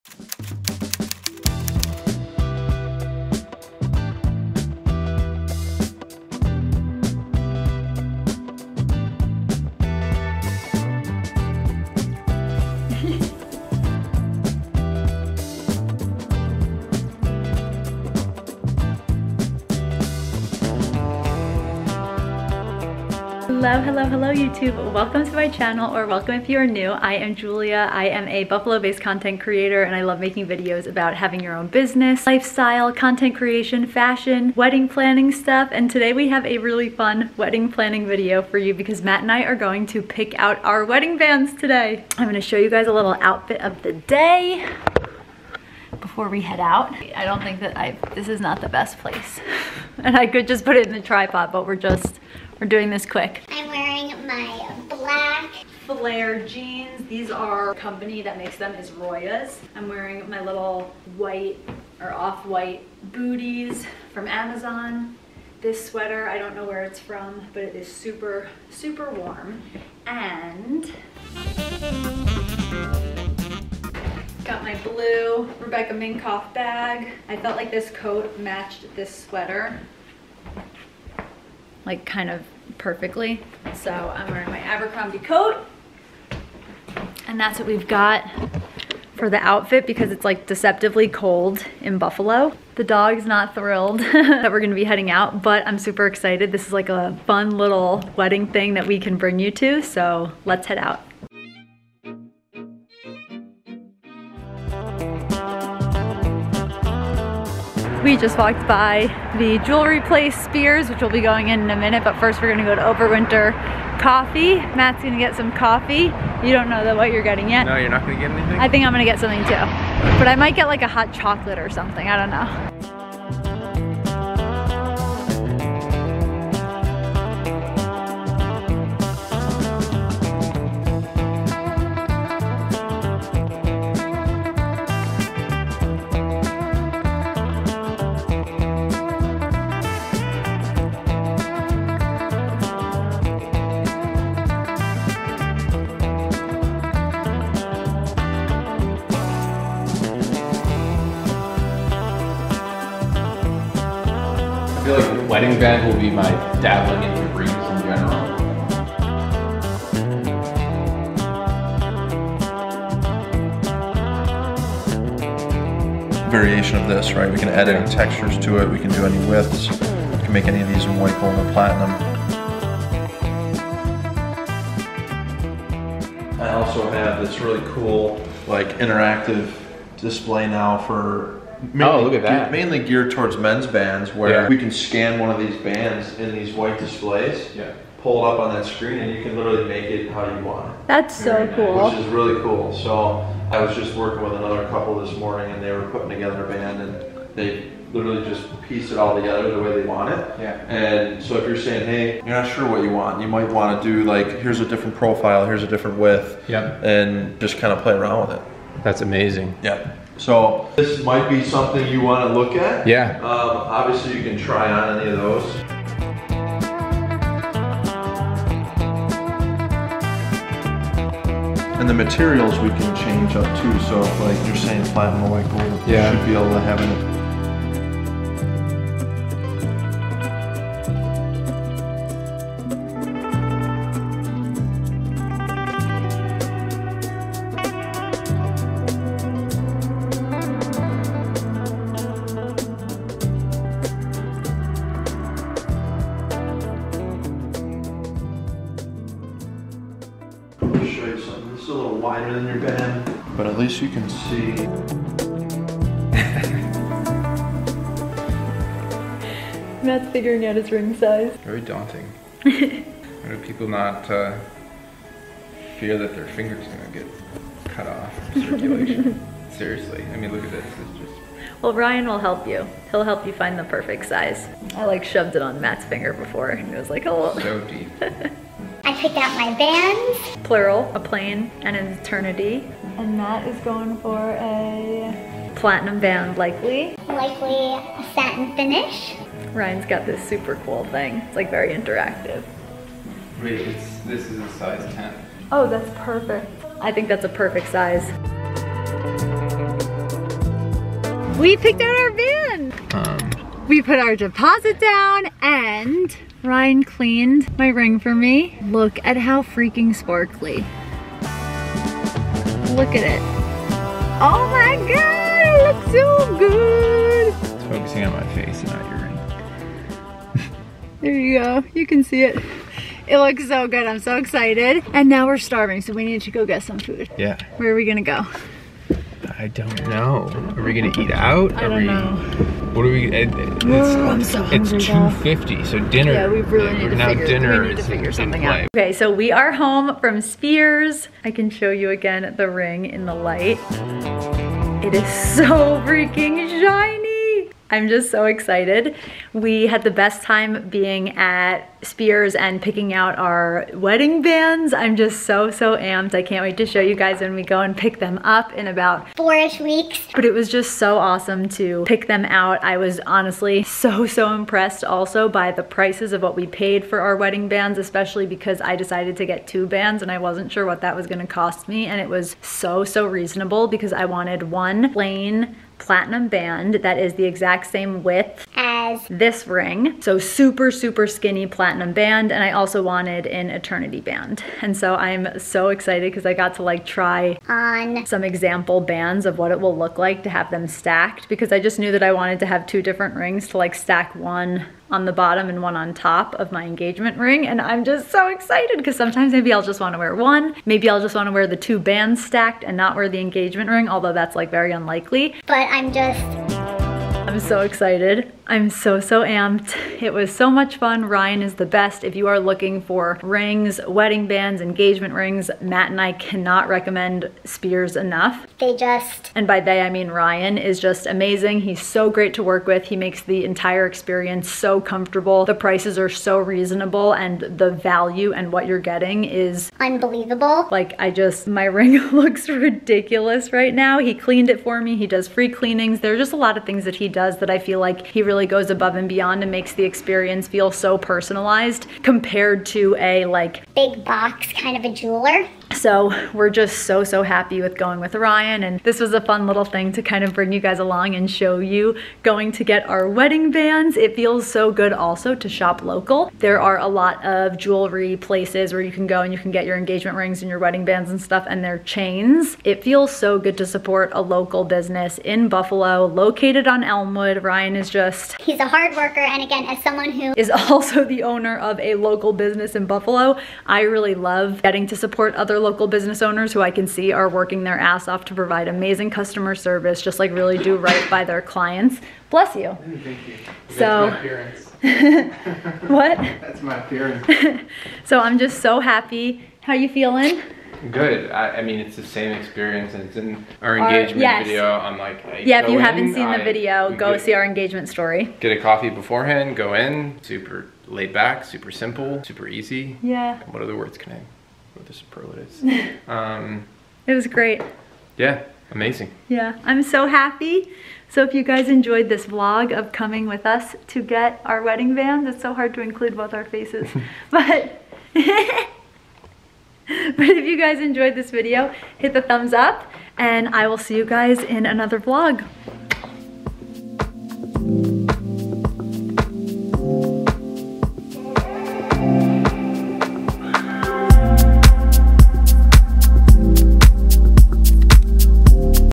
Wedding Planning: Picking Out Our Wedding Bands At Spears In Buffalo, NY
Come along with us as we do some wedding planning during the weekend and spend time picking out our wedding bands at the jeweler that we got my engagement ring at, Spears Co in Buffalo. It's located on Elmwood Village and we have absolutely loved working with Ryan on both my engagement and our wedding bands!

Let me know what you think of our final decisions!

CAMERA GEAR:
Camera 1: Canon G7X PowerShot
Camera 2: iPhone 11

Jules